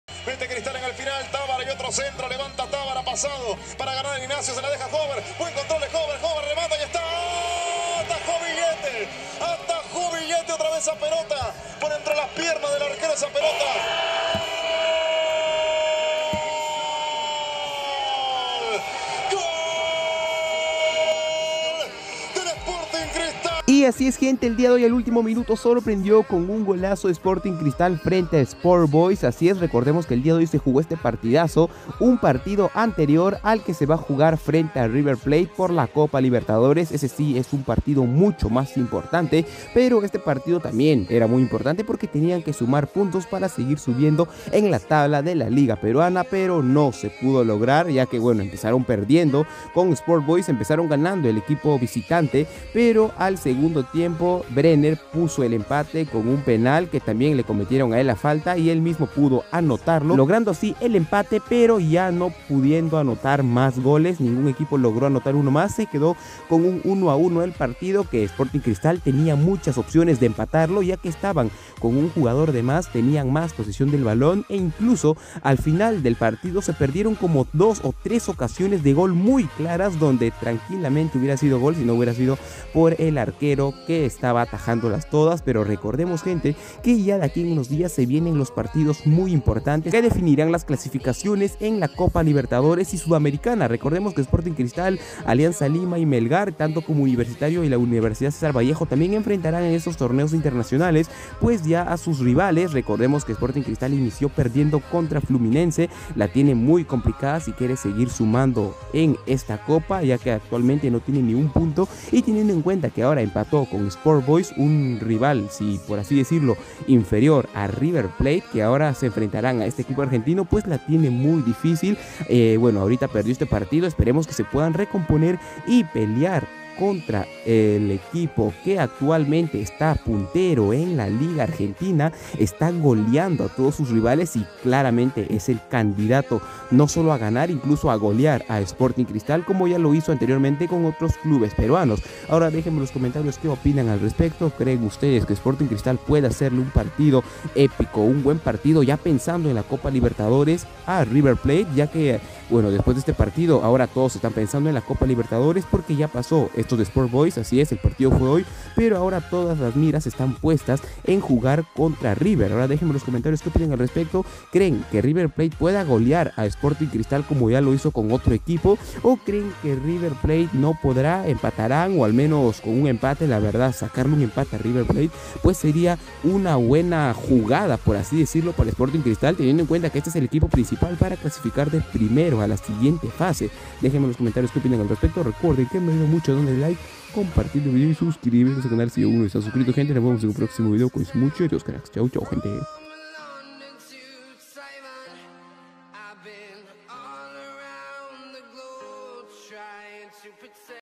BRUTAL REMONTADA CON DOS GOLAZOS AL ÚLTIMO MINUTO DE SPORTING CRISTAL HOY VS SPORT BOYS SORPRENDIÓ
Peruano sporting cristal remonta partidazo vs sport boys sorprende
Jugador peru sporting cristal vs sport boys resumen